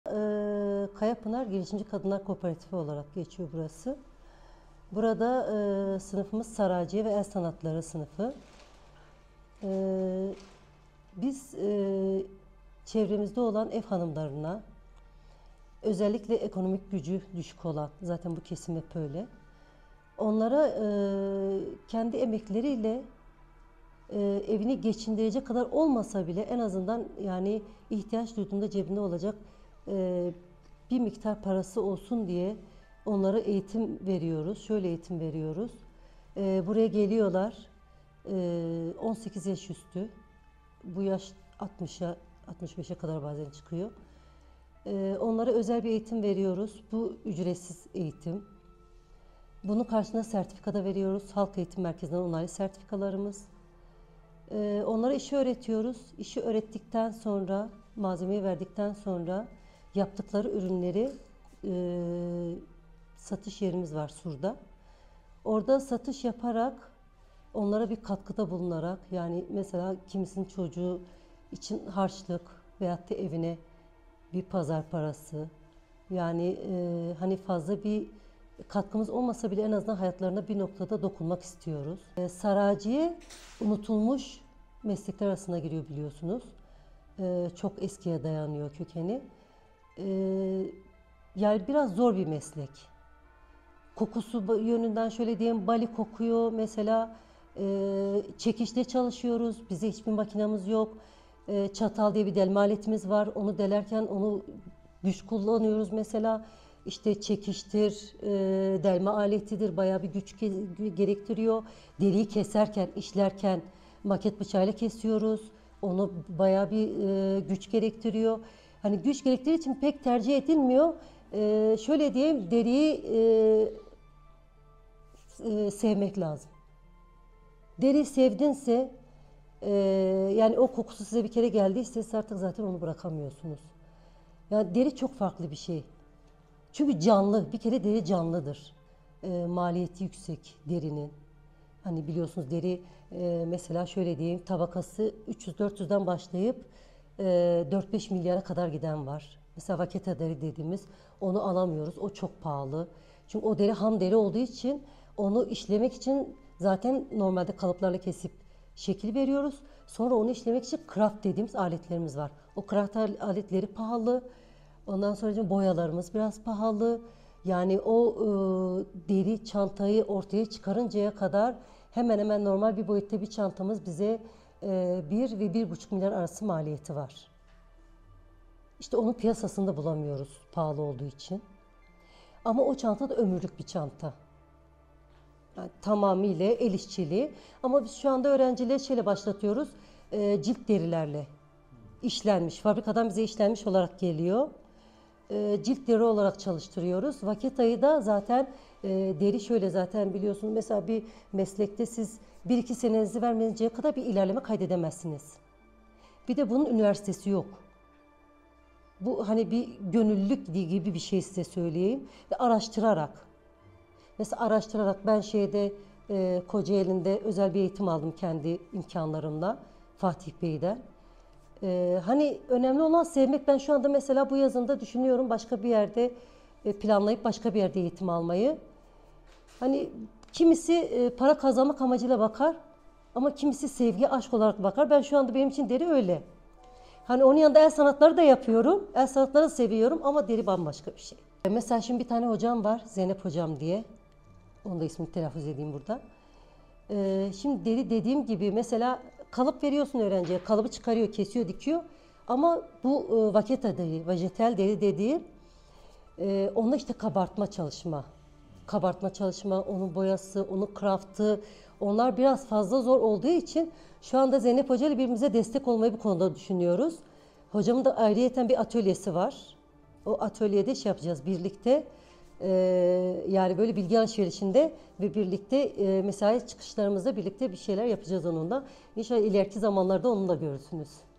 Tamamen El Yapımı Deri işlemesi
Asuman Karakaş'ın tamamen el yapımı deri ustaların çok az sayıda kaldığını belirterek bu meslek zanaatın inceliklerin özgünlüğüne olan vurguyu yansıttığını belirtti.